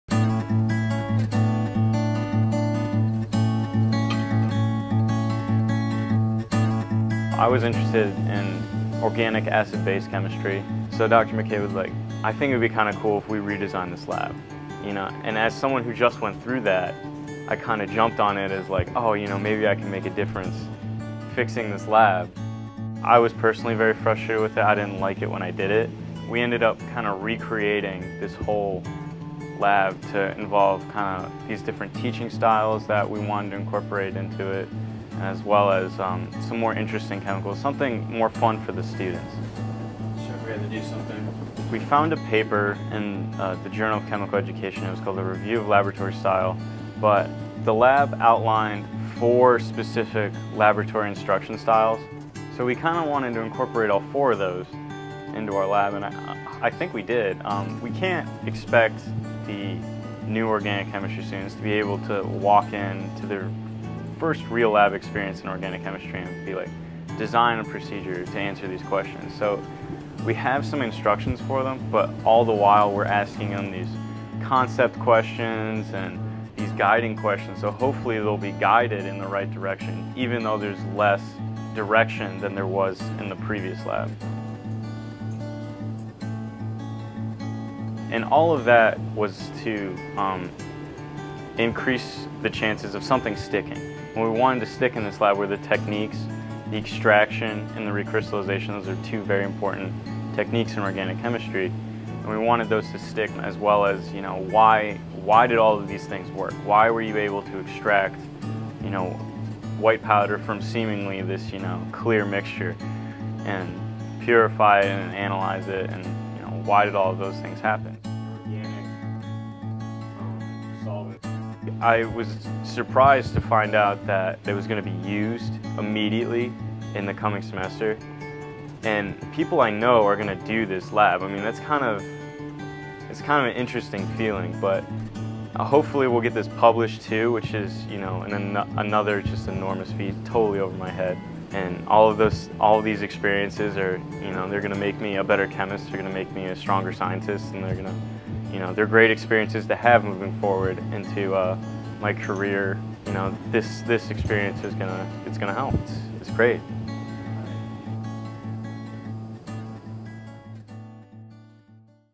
Student Research: Organic Chemistry Lab Procedure
Matt McErlean '12 thought the organic chemistry lab procedure could be improved. So he did something about it—he rewrote it.